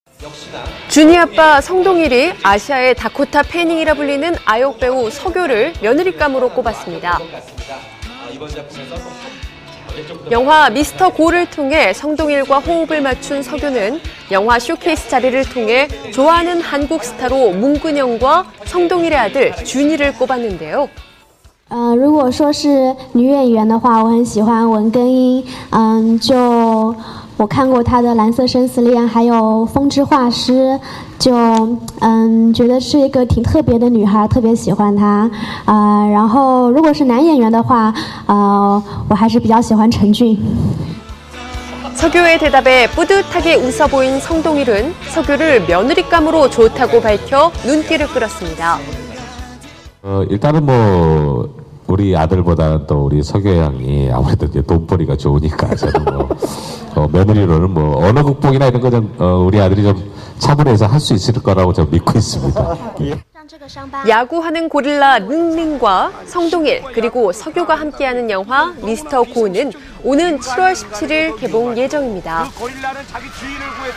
[Y-STAR] Sung Dongil, "I hope Xu Jiao would be my daughter-law-in" (영화 [미스터 고]의 성동일, '아들 며느리감은 서교')
'준이 아빠' 성동일이 아시아의 다코타 패닝이라 불리는 아역배우 서교를 며느릿감으로 꼽았습니다.

영화 [미스터 고]를 통해 성동일과 호흡을 맞춘 서교는 영화 쇼케이스 자리를 통해 좋아하는 한국 스타로 문근영과 성동일의 아들 준이를 꼽았는데요.

[인터뷰: 서교]

Q) 좋아하는 한국 스타가 있다면?

A) 만약에 여배우 중에 고르라고 한다면 저는 문근영씨를 너무 좋아합니다 [가을동화]도 재밌게 봤었고 [바람의 화원]도 너무 재밌게 봤어고요 작품을 보면서너무 좋아하는 배우고요 남자배우 중에 만약에 선택하라고 하신다면 저는 성준!

서교의 대답에 뿌듯하게 웃어 보인 성동일은 서교를 며느릿감으로 좋다고 밝혀 눈길을 끌었습니다.

[인터뷰:성동일]

Q) 서교가 준이를 좋아하는데?

A) 일단은 뭐 우리 아들보다는 서교양이 돈벌이가 좋으니까 며느리로는 언어극복이나 이런 건 우리 아들이 차분해서 할 수 있을 거라고 믿고 있습니다

야구하는 고릴라 링링과 성동일 그리고 서교가 함께 하는 영화 [미스터 고]는 오는 7월 17일 개봉 예정입니다.